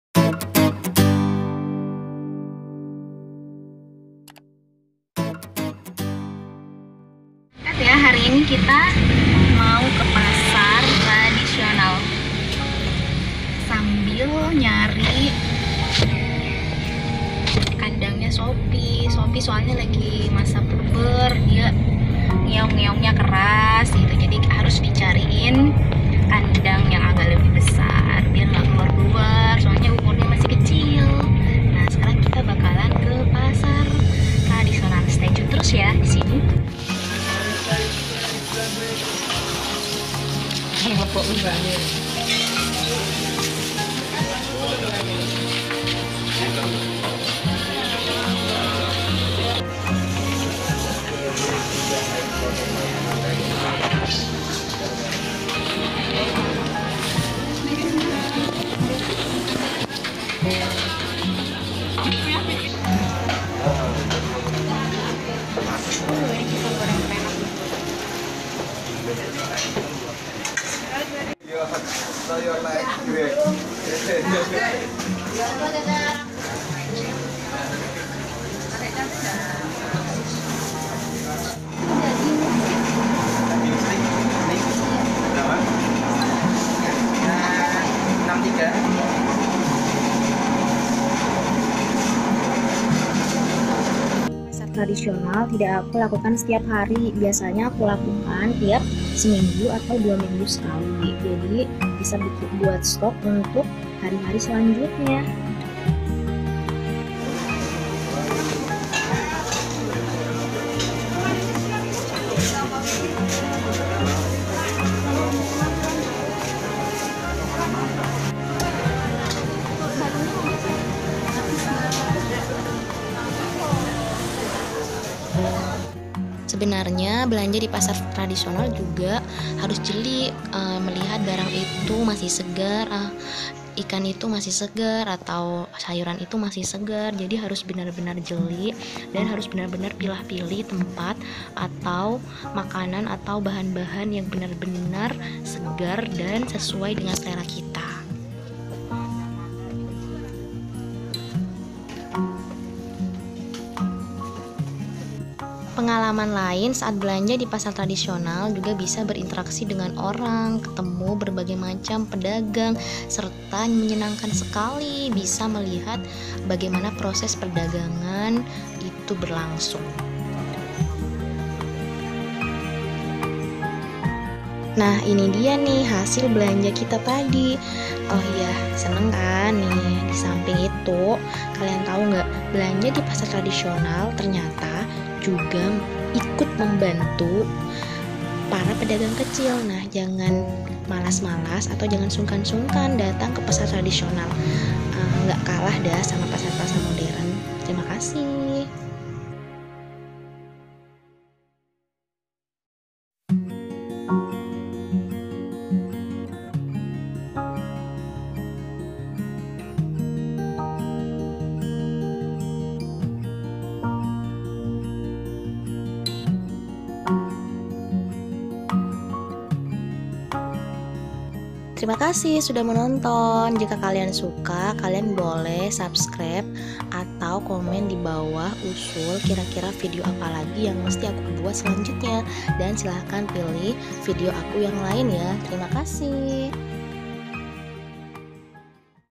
BELANJA BELANJI DI PASAR I VLOG
Ikutin aku belanja di pasar tradisional yuk!